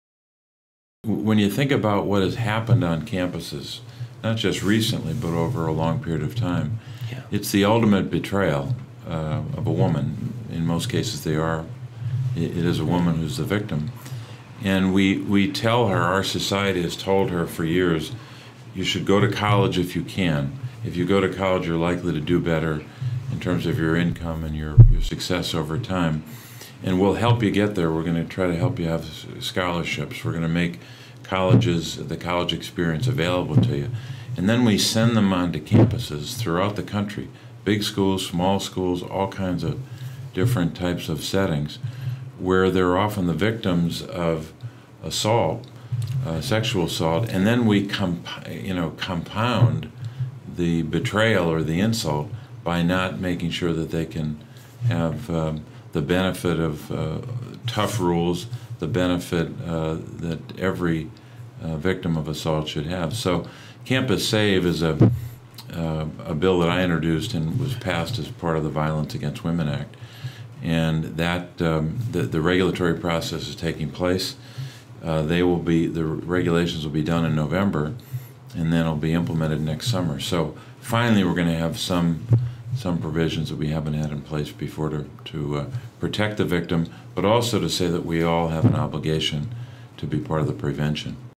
StateViews: U.S. Senator Bob Casey on "The Ultimate Betrayal"
This is an excerpt from StateViews, a segment from The Daily Collegian that gives members of the Pennsylvania and Penn State community a chance to share their opinions and views. This week, United States Sen. Bob Casey sat down with Opinions Editor Mitchell Culler to discuss his opinions on political topics.